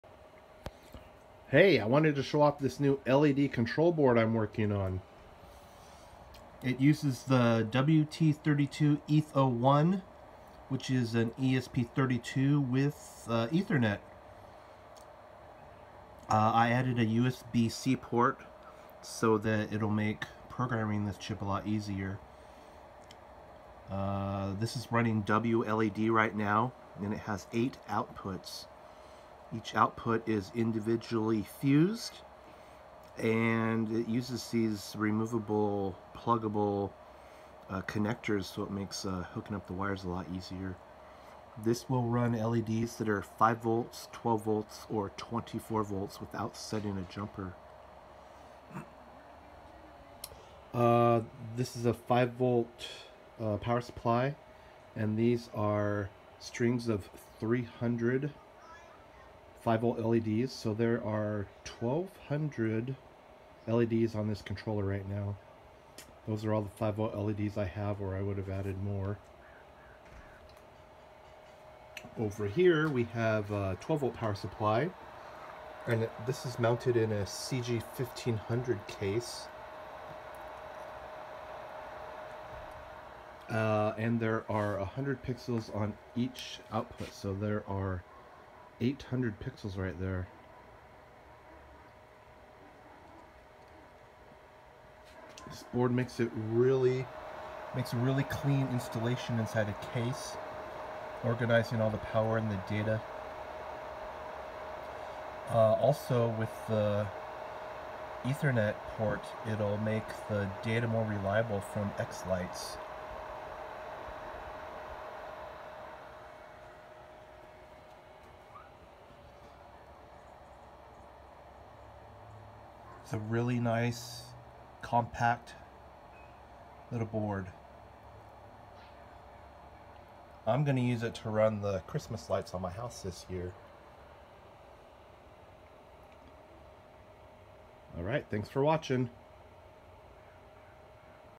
8 port ESP32 LED distro board running WLED with ethernet and 5v, 12v, or 24v input power
This is a fused 8 port LED board running WLED on an WT32-ETH01. It has an Ethernet port and accepts 5v, 12v or 24v input power. Also includes a USB-C port for updating the software.

I wanted to make a simple to use board for running LEDs in a home Christmas light display. Being able to distribute both data and power was a must. It really needed an ethernet port for more reliable data transfers from xLights. I also wanted it to accept multiple voltage inputs with out having to set jumpers. The pluggable terminal block connectors make it really simple to set up.